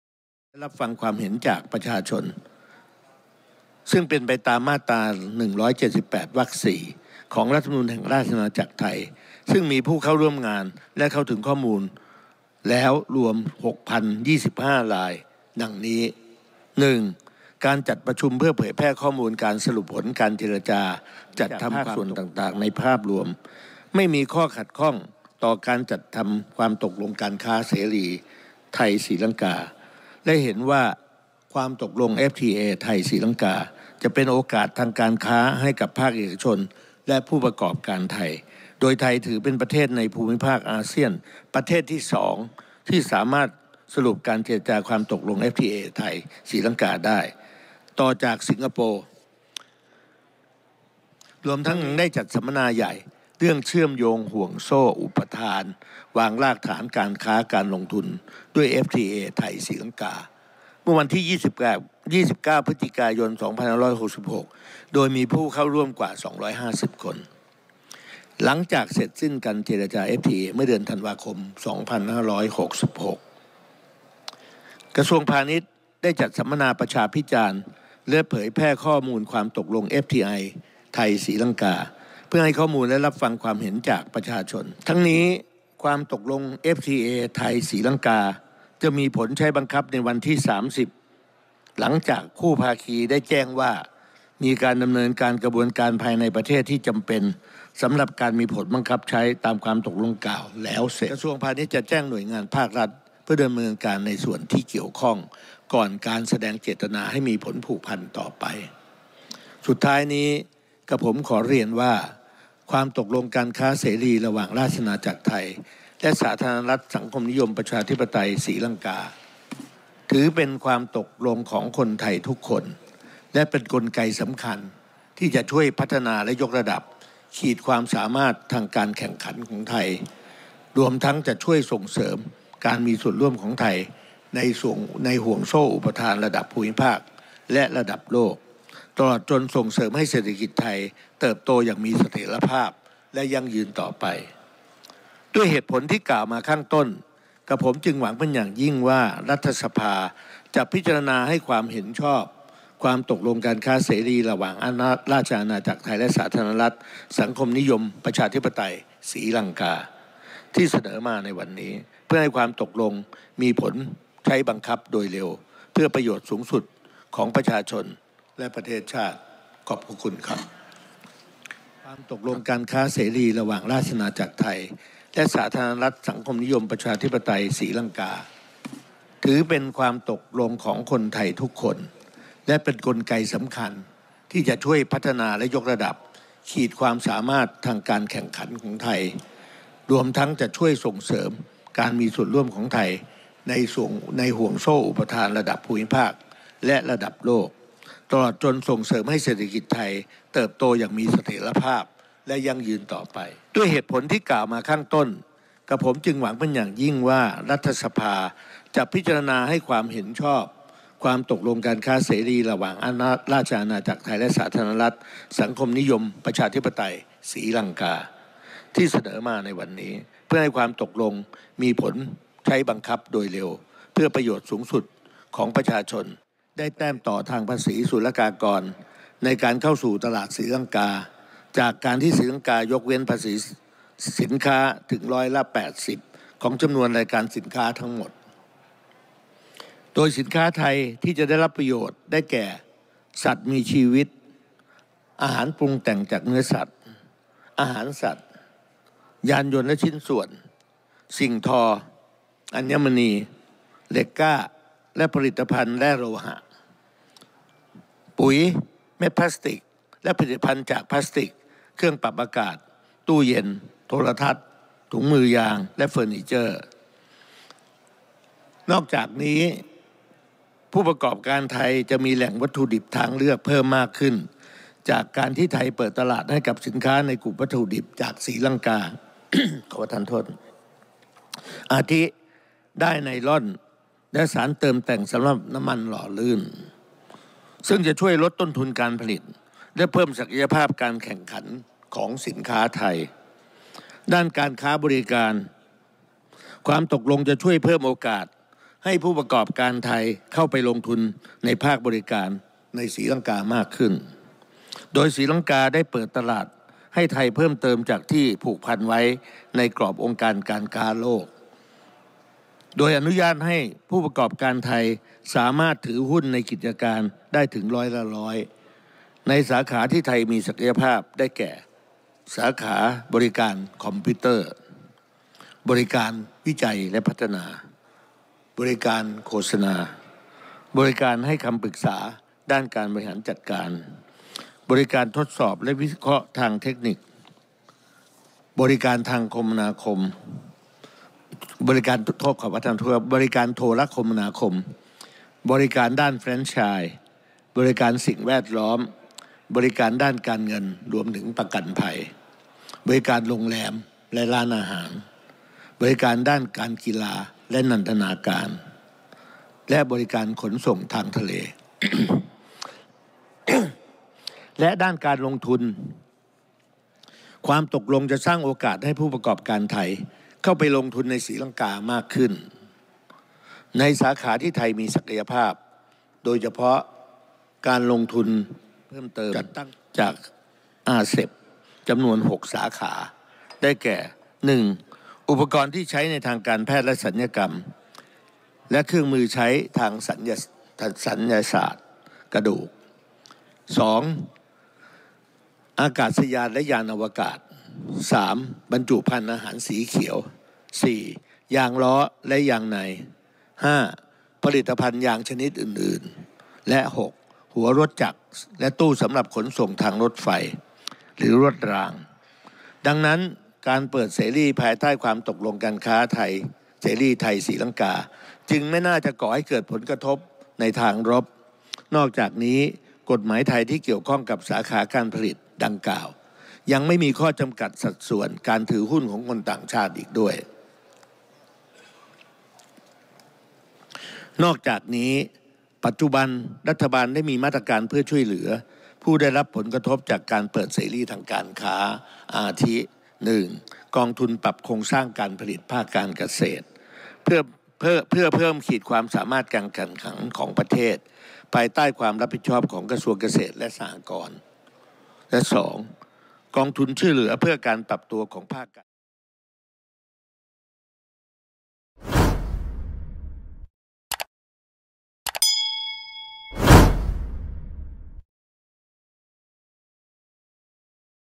“ภูมิธรรม” มั่นใจดันผู้ประกอบการเพิ่มแต้มต่อการค้า เปิดประตูสู่เอเชียใต้ หลังดัน FTA ไทย – ศรีลังกา
“ภูมิธรรม” มั่นใจดันผู้ประกอบการเพิ่มแต้มต่อการค้า เปิดประตูสู่เอเชียใต้ หลังดัน FTA ไทย – ศรีลังกา ผ่านสภาสำเร็จ

วันที่ 29 มีนาคม 2567 เวลา 13.00 น.ที่รัฐสภา นายภูมิธรรม เวชยชัย รองนายกรัฐมนตรีและรัฐมนตรีว่าการกระทรวงพาณิชย์ เปิดเผยว่า ที่ประชุมร่วมกันของรัฐสภา ครั้งที่ 1 (สมัยสามัญประจำปีครั้งที่ 2) ได้มีมติเห็นชอบความตกลงการค้าเสรีไทย – ศรีลังกา ตามที่คณะรัฐมนตรีเสนอ ทำให้ FTA ไทย – ศรีลังกา เป็นความตกลงการค้าเสรีฉบับที่ 15 ของไทย และเป็นความตกลงการค้าเสรีฉบับแรกของรัฐบาลภายใต้การนำของนายกรัฐมนตรีเศรษฐา ทวีสิน ซึ่งความตกลง FTA ไทย - ศรีลังกา จะเป็นกลไกสำคัญในการเพิ่มขีดความสามารถทางการแข่งขันให้กับผู้ประกอบการไทยในตลาดศรีลังกา รวมทั้งขยายโอกาสด้านการค้าและการลงทุนให้กับนักลงทุนไทยที่ต้องการเป็นส่วนหนึ่งของห่วงโซ่อุปทานในภูมิภาคเอเชียใต้ เนื่องจากศรีลังกาเป็นประเทศที่มีจุดแข็งด้านที่ตั้งทางยุทธศาสตร์อยู่บนเส้นทางเดินเรือที่สำคัญของโลกในมหาสมุทรอินเดีย เชื่อมต่อภูมิภาคเอเชียตะวันออก และเอเชียใต้ สู่ตะวันออกกลาง แอฟริกา และยุโรป รวมทั้งยังมีวัตถุดิบทั้งด้านการเกษตร ประมง และแร่ธรรมชาติ ที่เหมาะสมสำหรับการเป็นฐานการลงทุนของนักลงทุนไทย

โดยในด้านการค้าสินค้า ความตกลงฯ จะช่วยขยายโอกาสให้สินค้าไทยได้แต้มต่อทางภาษีศุลกากรในการเข้าสู่ตลาดศรีลังกาจากการที่ศรีลังกายกเว้นภาษีสินค้า ถึงร้อยละ 85 ของจำนวนรายการสินค้าทั้งหมด โดยสินค้าไทยที่จะได้รับประโยชน์ ได้แก่ สัตว์มีชีวิต อาหารปรุงแต่งจากเนื้อสัตว์ อาหารสัตว์ ยานยนต์และชิ้นส่วน สิ่งทอ อัญมณี เหล็กกล้าและผลิตภัณฑ์ แร่โลหะ ปุ๋ย เม็ดพลาสติกและผลิตภัณฑ์จากพลาสติก เครื่องปรับอากาศ ตู้เย็น โทรทัศน์ ถุงมือยาง และเฟอร์นิเจอร์ นอกจากนี้ ผู้ประกอบการไทยจะมีแหล่งวัตถุดิบทางเลือกเพิ่มมากขึ้น จากการที่ไทยเปิดตลาดให้กับสินค้าในกลุ่มวัตถุดิบจากศรีลังกา อาทิ ด้ายไนลอน และสารเติมแต่งสำหรับน้ำมันหล่อลื่น ซึ่งจะช่วยลดต้นทุนการผลิตและเพิ่มศักยภาพการแข่งขันของสินค้าไทย ด้านการค้าบริการ ความตกลงฯ จะช่วยเพิ่มโอกาสให้ผู้ประกอบการไทยเข้าไปลงทุนในภาคบริการในศรีลังกามากขึ้น โดยศรีลังกาได้อนุญาตให้ผู้ประกอบการไทยสามารถถือหุ้นในกิจการได้ถึงร้อยละ 100 ในสาขาที่ไทยมีศักยภาพ ได้แก่ สาขาบริการคอมพิวเตอร์ บริการวิจัยและพัฒนา บริการโฆษณา บริการให้คำปรึกษาด้านการบริหารจัดการ บริการทดสอบและวิเคราะห์ทางเทคนิค บริการโทรคมนาคม บริการด้านแฟรนไชส์ บริการสิ่งแวดล้อม บริการด้านการเงินและประกันภัย บริการโรงแรมและร้านอาหาร บริการด้านการกีฬาและนันทนาการ และบริการขนส่งทางทะเล และด้านการลงทุน ความตกลงฯ จะสร้างโอกาสให้ผู้ประกอบการไทยเข้าไปลงทุนในศรีลังกามากขึ้น ในสาขาที่ไทยมีศักยภาพ โดยเฉพาะการลงทุนจัดตั้งธุรกิจในสาขาการผลิต จำนวน 35 สาขา อาทิ การแปรรูปอาหาร การผลิตสิ่งทอ การผลิตยานยนต์และชิ้นส่วนยานยนต์ การผลิต อุปกรณ์อิเล็กทรอนิกส์ และการผลิตเครื่องมือและอุปกรณ์ทางการแพทย์และทันตกรรม นอกจากนี้ นักลงทุนไทยจะได้รับการคุ้มครองการลงทุน อาทิ กรณีการลงทุนที่ได้รับผลกระทบจากการเวนคืน หรือเหตุการณ์ไม่สงบภายในประเทศ รวมถึงนักลงทุนไทยยังสามารถใช้กลไกการระงับข้อพิพาทระหว่างนักลงทุนกับรัฐได้อีกด้วย

นายภูมิธรรม เผยว่า หลังจากนี้ กระทรวงพาณิชย์จะแจ้งหน่วยงานภาครัฐเพื่อดำเนินการในส่วนที่เกี่ยวข้อง ก่อนแสดงเจตนาต่อศรีลังกาให้ความตกลงฯ มีผลผูกพัน เพื่อให้ FTA ไทย – ศรีลังกา มีผลบังคับใช้โดยเร็ว รวมทั้งจะเดินหน้าประชาสัมพันธ์และเผยแพร่ข้อมูลและประโยชน์ที่ไทยได้รับให้ถึงกลุ่มผู้ประกอบการในวงกว้าง โดยเฉพาะ MSMEs ให้ทราบถึงโอกาสและสามารถใช้ประโยชน์จากความตกลงฯ ได้อย่างเต็มที่ และจะช่วยให้ผู้ประกอบการรายใหม่สามารถเข้าสู่ตลาดการค้าระหว่างประเทศได้เพิ่มมากขึ้น

ทั้งนี้ ในปี 2566 การค้าระหว่างไทยและศรีลังกามีมูลค่า 415.30 ล้านดอลลาร์สหรัฐ โดยไทยส่งออกไปศรีลังกา 291.17 ล้านดอลลาร์สหรัฐ และนำเข้าจากศรีลังกา 124.13 ล้านดอลลาร์สหรัฐ สินค้าส่งออกสำคัญของไทย อาทิ อัญมณีและเครื่องประดับน้ำมันสำเร็จรูป ผ้าผืน ยางพารา เคมีภัณฑ์ และเม็ดพลาสติก สินค้านำเข้าสำคัญจากศรีลังกา อาทิ เครื่องเพชรพลอย อัญมณี เงินแท่งและทองคำ เสื้อผ้าสำเร็จรูป เครื่องจักรไฟฟ้าและส่วนประกอบ พืชและผลิตภัณฑ์จากพืช และกาแฟ ชา เครื่องเทศ